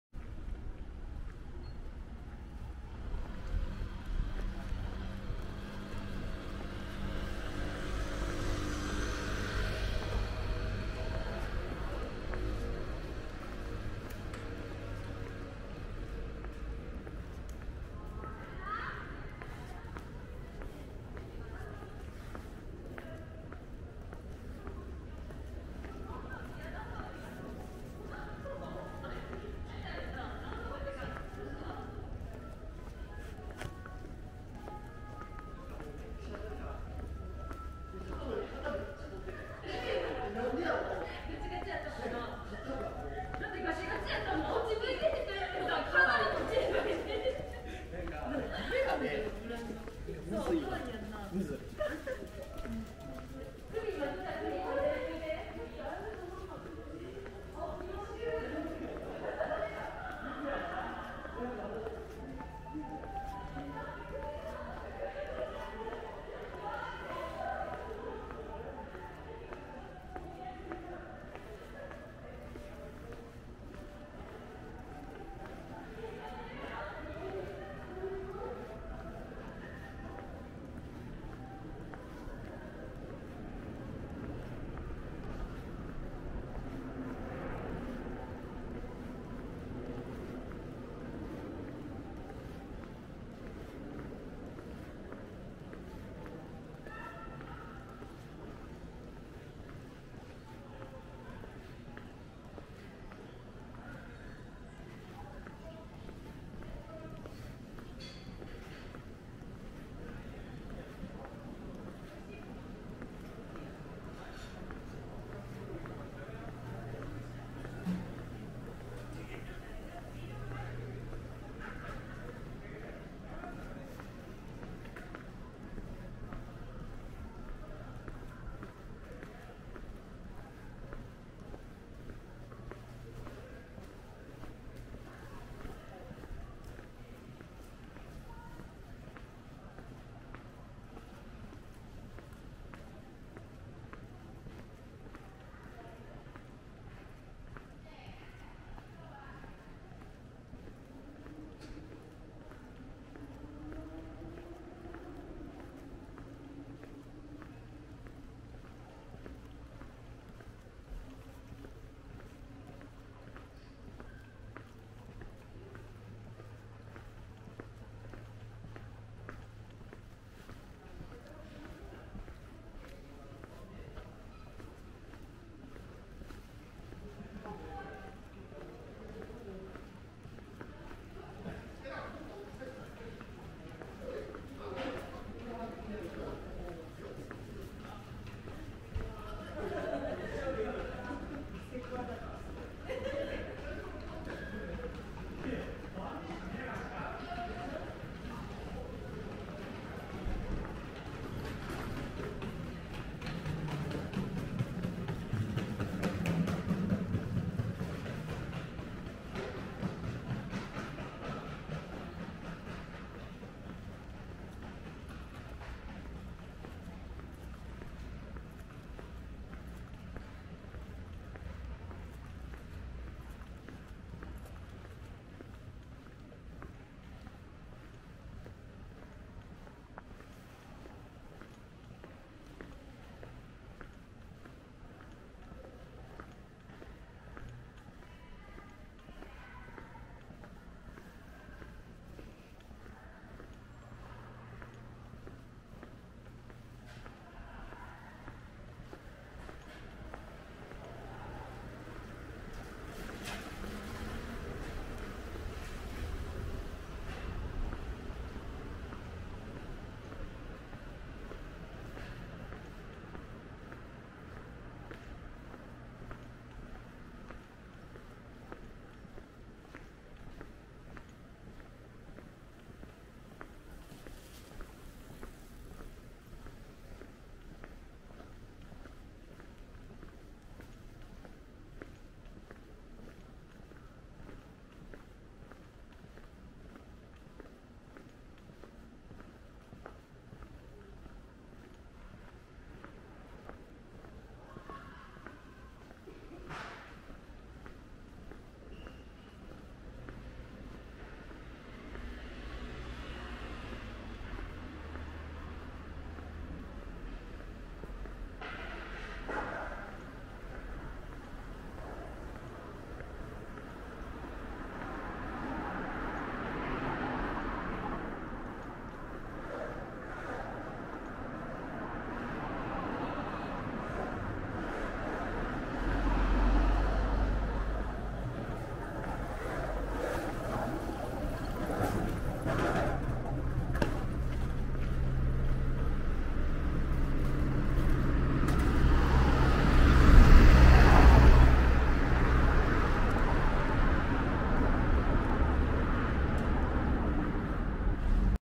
散歩 天五中崎通商店街
天五中崎通商店街を散歩しました。

大阪市、北区、天満駅、天神橋駅、中崎町駅、梅田、黒崎町、浪速町、都島通、おいでやす通り、2021年5月撮影